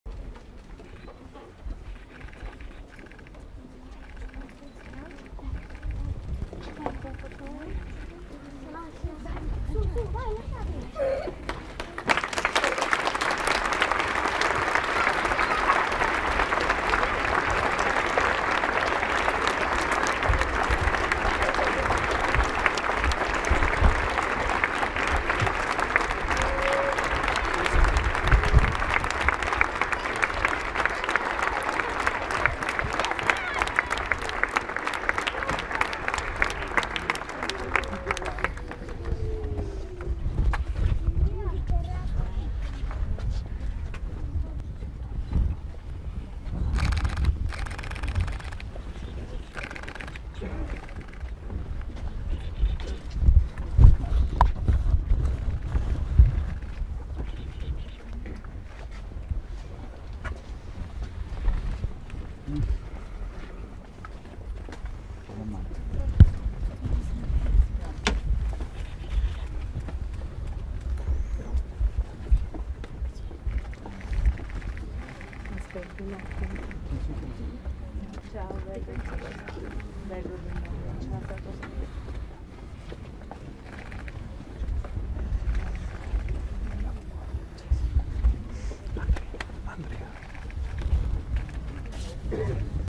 Marche, applausi e lacrime per l'ultimo saluto al piccolo Mattia Luconi
L'uscita del feretro dalla chiesa di Santa Maria Assunta, a Barbara in provincia di Ancona, per i funerali del piccolo Mattia Luconi. Il bambino di otto anni morto nell'alluvione delle Marche.  L'ARTICOLO Alluvione nelle Marche, in migliaia ai funerali del piccolo Mattia Luconi. Don Paolo: "E' in mezzo a noi"